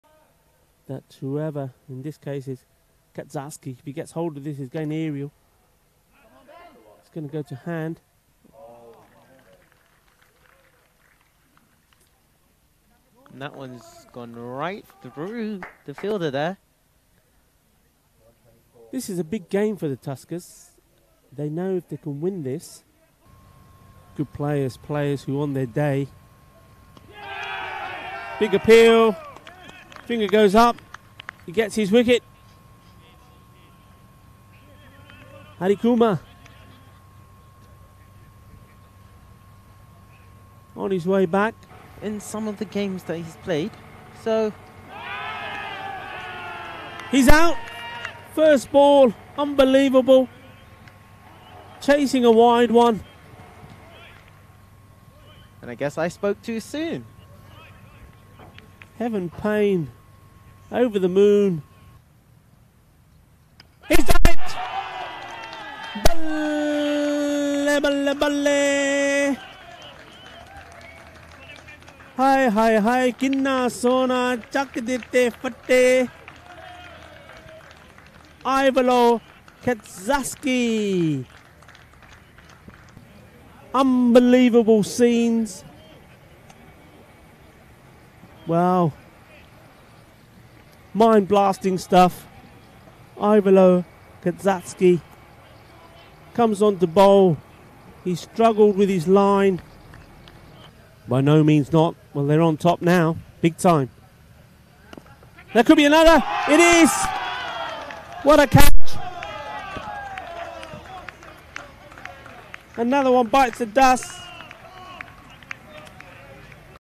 Boom! Bulgaria finest Ivaylo Katzarski in the wickets
Man of the Match: Ivaylo Katzarski 12(21) & (4/16) - INDT vs BCC | Highlights | European Cricket Series Bulgaria Day 4 | Bulgaria 2020